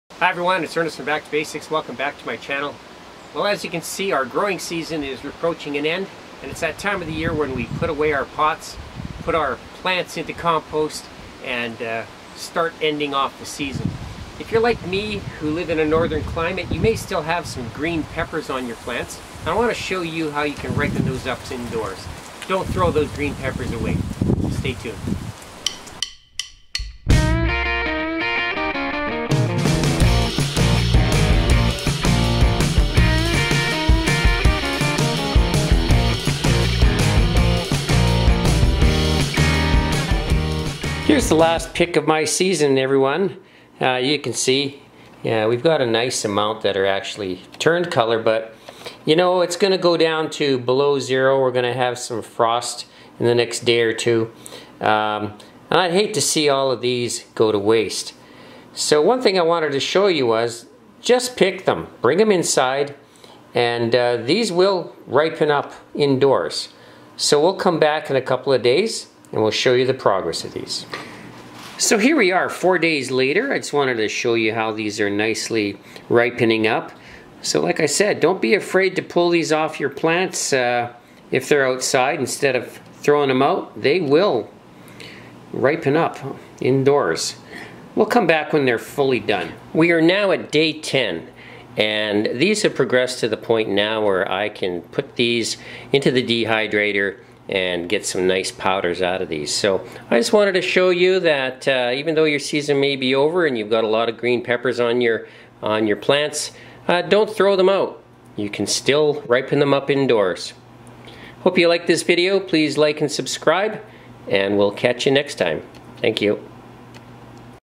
Ultra Hot Pepper Project 2017 Part 9 - Ripen Your Peppers Indoors
Ultra Hot Pepper Project 2017 Part 9 - Do you still have green peppers on your plants and frost is approaching fast? Don't let those pods go to waste! Pick them and bring them indoors. Place them in a bowl and just leave them on your kitchen counter. They will still ripen.